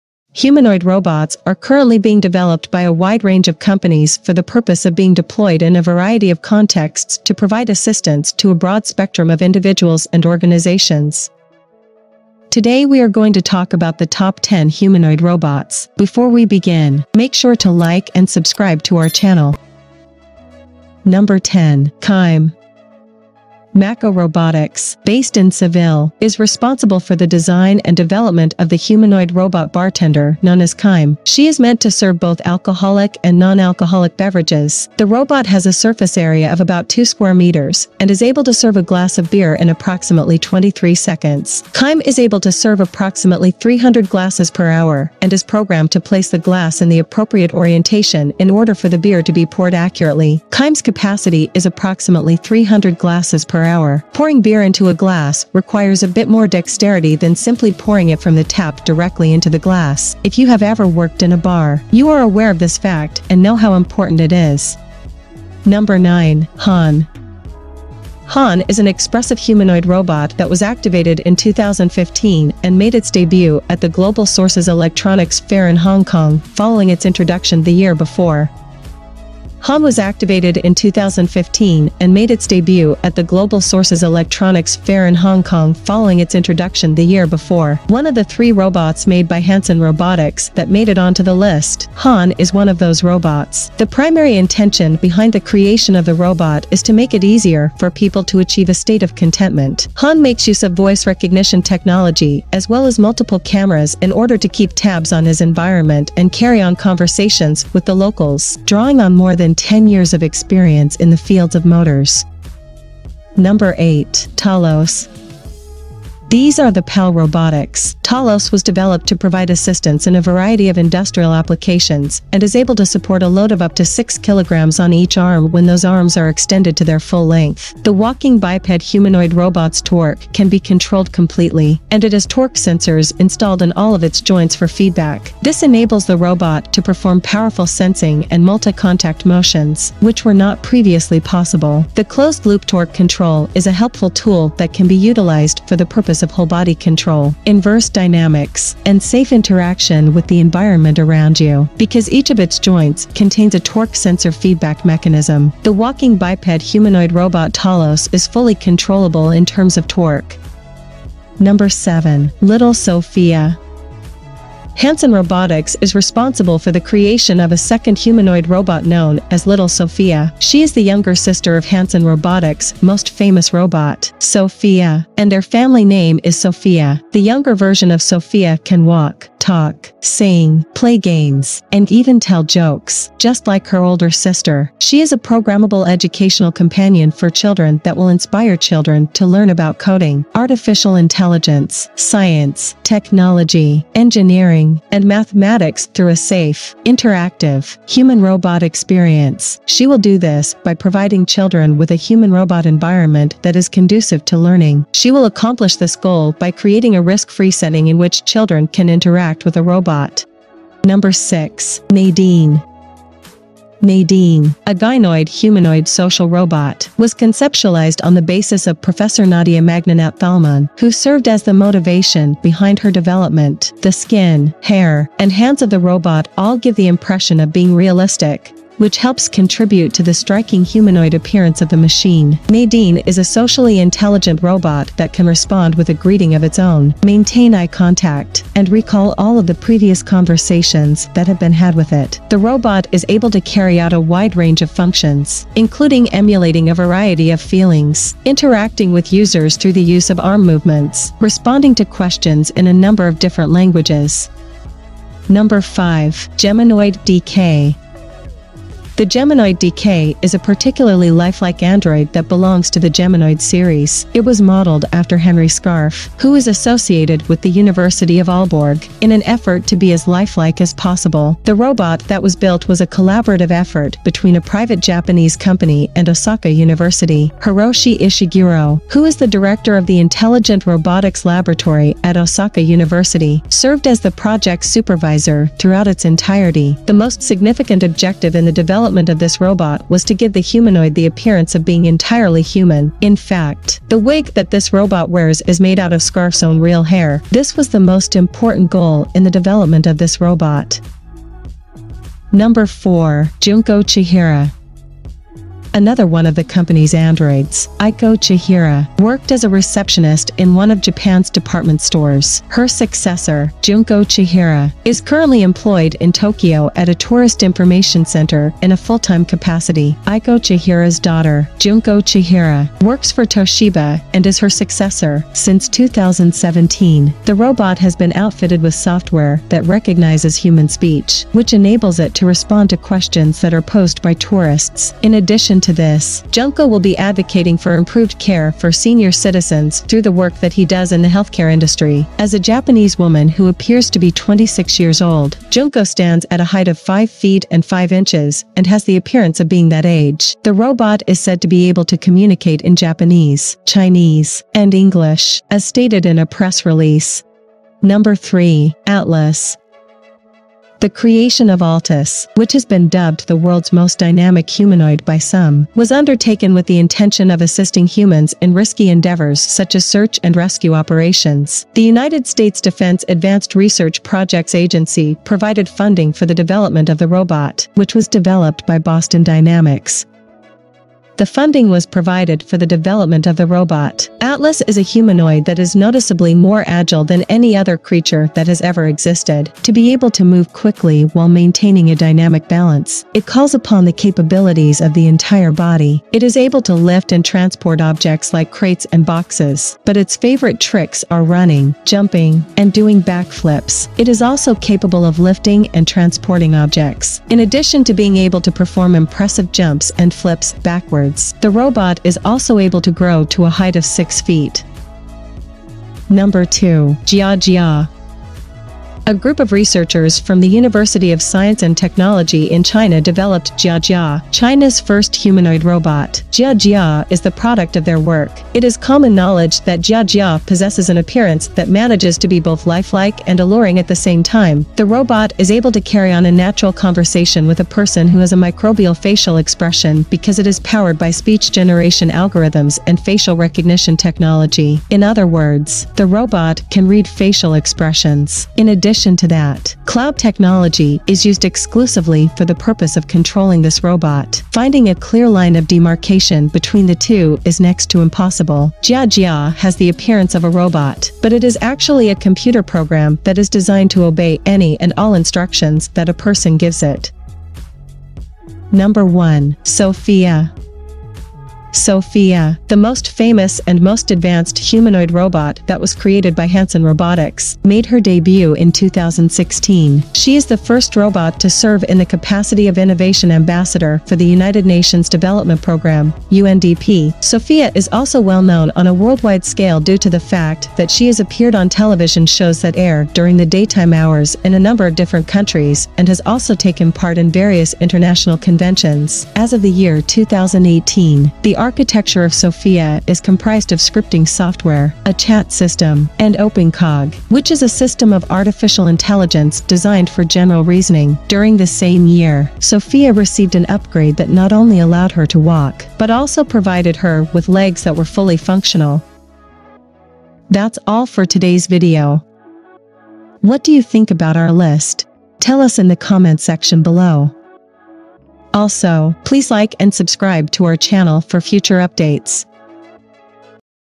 Top 10 Most Advanced Humanoid Robots Of 2022 (Insanely Shocking)
Top 10 Most Advanced Humanoid Robots of 2022: In this video, I explore and speculate about the top 10 most advanced humanoid robots of 2022.

Artificial Intelligence has become a cornerstone of global tech, with several AI-powered robots poised to join the workforce in the coming years.

From AI robots to walking humanoid helpers, here's a roundup of the most technologically advanced robotic creations with which we'll share our planet.

VIDEO CHAPTERS:

00:00 Introduction
00:21 Macco Robotics Beer Pouring Kime Robot
01:06 Han - The Robot Head
01:53 TALOS - The Walking Biped Humanoid Robot
02:42 Little Sophia - Hanson Robotics
03:31 Nadine - Gynoid Humanoid Social Robot
04:16 Geminoid DK  - Realistic Android Designed for Research into Human-Robot Interaction
05:03 Junko Chihira - Gynoid robot Developed by Toshiba, Japan
05:58 Atlas - Biapedal Humanoid Robot Developed by Boston Dynamics
06:56 Jia Jia - China's 1st Interactive Robot
07:49 Sophia - Social Humanoid Robot Developed by the Hong Kong-based Company Hanson Robotics
08:40 Watch This Next Videos

Top 10 High-level Technologies of 2022 That Change the World

Latest Farm Technology In 2022 (Next Level)